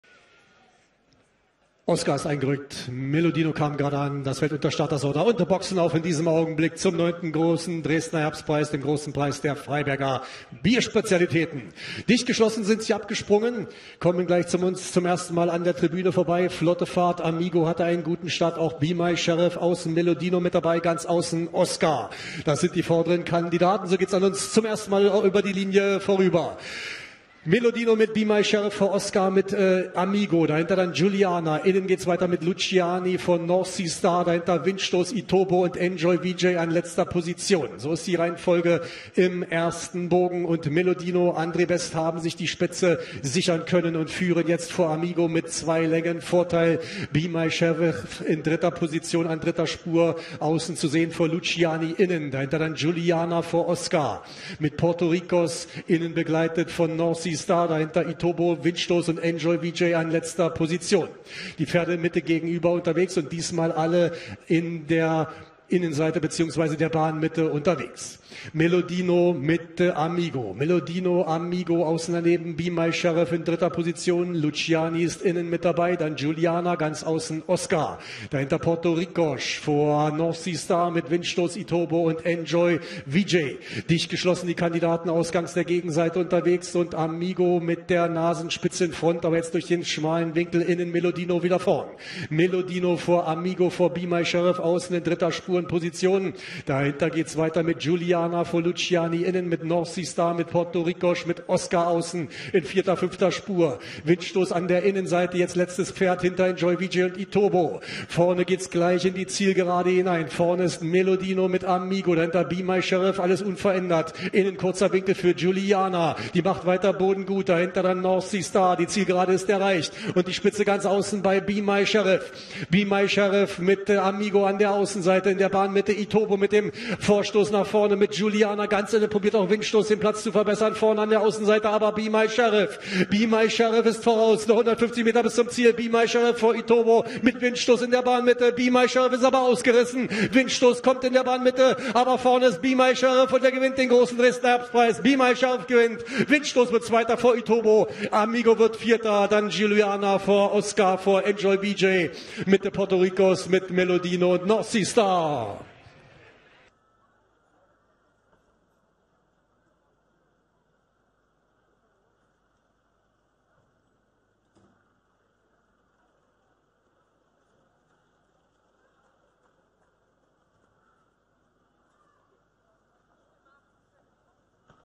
Dresden: Großer Preis der Freiberger Bierspezialitäten (L) - Sieger: Be My Sheriff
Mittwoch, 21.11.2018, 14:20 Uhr
Großer Preis der Freiberger Bierspezialitäten (9. Grosser Dresdner Herbstpreis)
Listenrennen (A), 2.200 m, 25.000 €, Boden: gut bis weich
Für 3-jährige und ältere Pferde., Gew. 55,5 kg. f.3j., 57,0 kg. f.4j.u.ält. Pferden, die seit 1.1.2018 ein Listen-Rennen gewonnen haben oder Zweiter oder Dritter in einem Gruppe-Rennen waren 1,5 kg, die seitdem mehrere Listen-Rennen oder ein Gruppe-Rennen gewonnen haben 3 kg mehr.

Geldpreise: 25.000 € (14.000, 6.500, 3.000, 1.500)., Ehrenpreis dem
Besitzer, Trainer, Reiter und Pfleger des Siegers.
250 € Einsatz (125, 125).

EINLAUF: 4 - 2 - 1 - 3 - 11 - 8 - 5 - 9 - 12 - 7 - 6

Pl. Name Nr. Box Abstand Gewinn Besitzer Trainer Reiter Gew. Quote
1. Be My Sheriff 4 5 überlegen 14.000 € Frau N.Siepmann H.Grewe L.Delozier 57,0 kg 88
2. Windstoß 2 4 4 3/4 Längen 6.500 € Gestüt Röttgen M.Klug M.Pecheur 58,5 kg 27
3. Itobo 1 7 Kopf 3.000 € Stall Totti H.-J.Gröschel W.Panov 58,5 kg 29
4. Amigo 3 2 1/2 Länge 1.500 € Rennstall Germanius Frau E.Fabianova J.Bojko 57,0 kg 200
5. Guiliana (FR) 11 6 3 1/4 Längen  Stall Torjäger Andr.Wöhler B.Murzabayev 55,5 kg 128
6. Oscar 8 12 2 1/2 Längen  R.Paulick D.Paulick M.Laube 57,0 kg 511
7. Enjoy Vijay 5 8 1 3/4 Längen  Gestüt Ittlingen P.Schiergen A.Starke 57,0 kg 67
8. Portorikos (IRE) 9 10 2 1/2 Längen  Autoopravovna Strnisko/Slowakei M.Stancik Jiri Palik 57,0 kg 353
9. Melodino
Sb. 12 11 1/2 Länge  Stall Heymann GmbH K.Demme A.Best 55,5 kg 164
10. Northsea Star 7 9 2 3/4 Längen  Gestüt Wittekindshof M.Klug R.Piechulek 57,0 kg 340
11. Luciani 6 1 49 Längen  Stall Hanse J.-P.Carvalho F.Minarik 57,0 kg 231
Be My Sheriff: s.u.

QUOTEN: Siegwette 88:10. - Platzwette 18, 13, 11:10. - Zweierwette 322:10. - Dreierwette 1.163:10. - Platz-Zwilling-Wette 48, 38, 17:10.
Auf Anordnung des Direktoriums wurde von Be My Sheriff eine Dopingprobe entnommen.
ZEIT DES RENNENS: 2:20,90     STARTZEIT: 14:24:00 Uhr     STARTER: 11     NICHTSTARTER: Sir Sun.